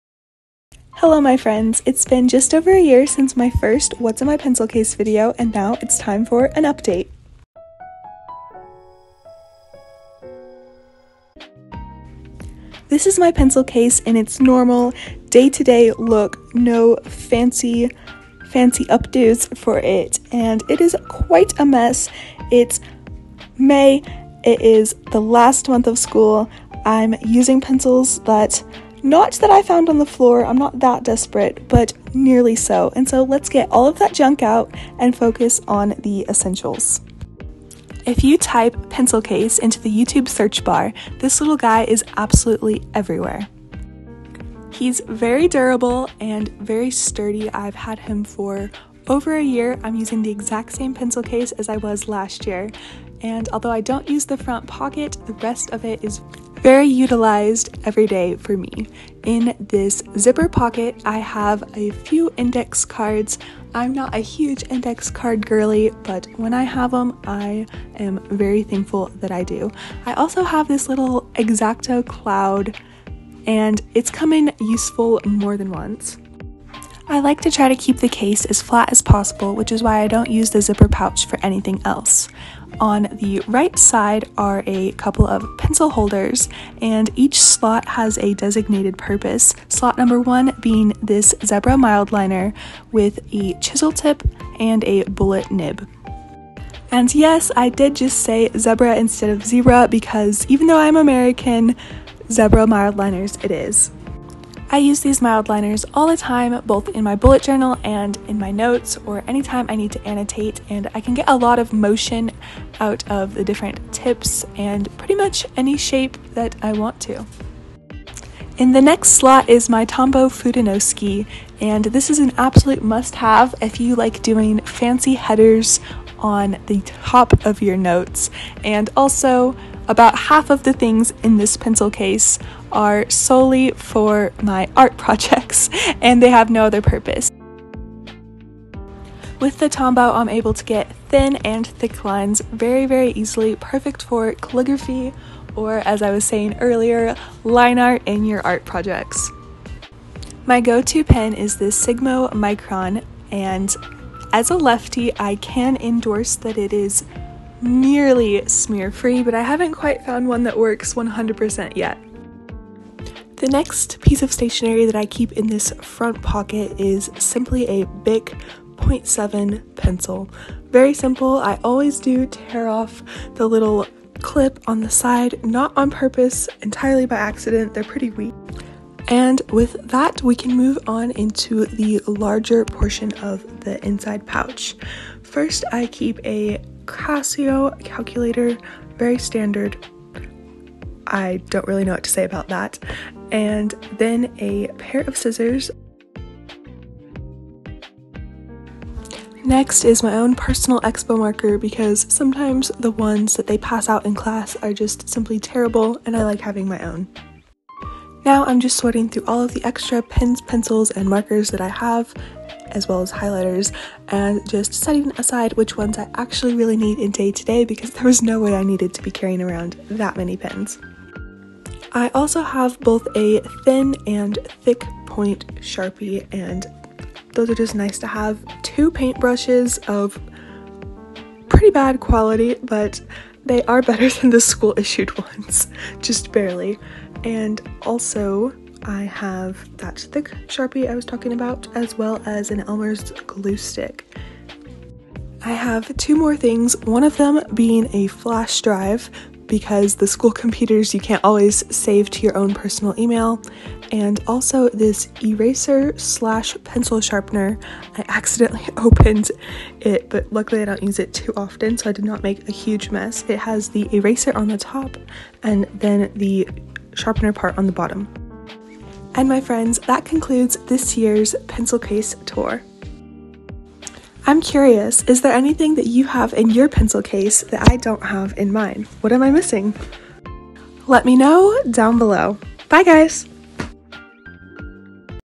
what's in my pencil case 2023
a little over a year ago, i made my first what's in my pencil case video. now, i'm making another one! i love these pens and markers and other various stationery supplies :D

also, i accidentally bought *THE* studytube pencil case haha